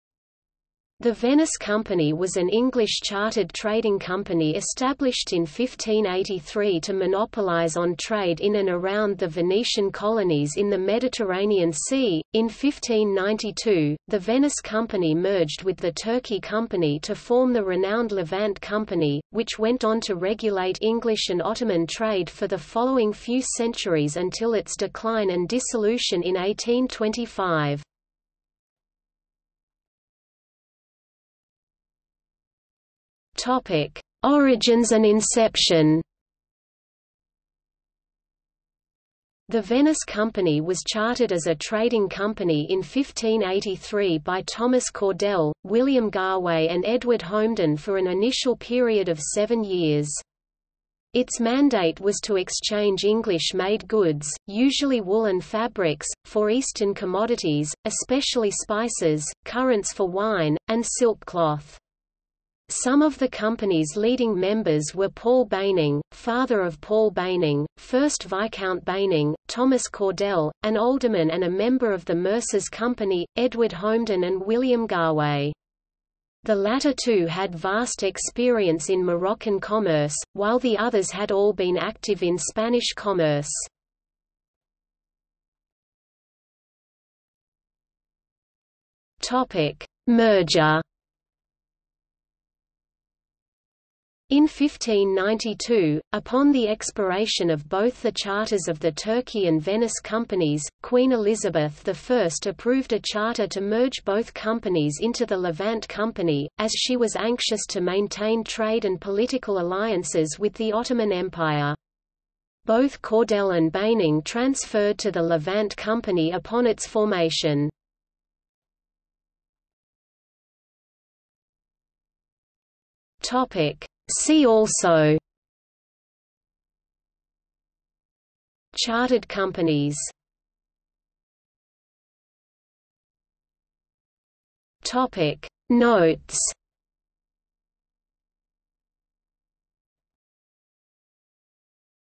Venice Company | Wikipedia audio article
00:00:29 1 Origins and inception
00:01:25 2 Merger
00:01:59 3 See also
00:02:11 4 Notes

- Socrates

SUMMARY
The Venice Company was an English chartered trading company established in 1583 to monopolise on trade in and around the Venetian colonies in the Mediterranean Sea.In 1592, the Venice Company merged with the Turkey Company to form the renowned Levant Company, which went on to regulate English and Ottoman trade for the following few centuries until its decline and dissolution in 1825.